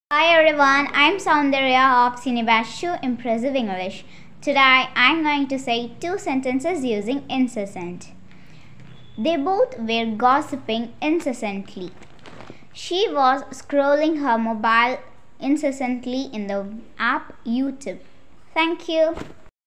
vocabulary development|impresivenglish|speak english with fluently |online class
Learning can be effective when it is applied in our every day conversations.
Soundarya of Senior Batch 2 is presenting two sentences using the word INCESSANT.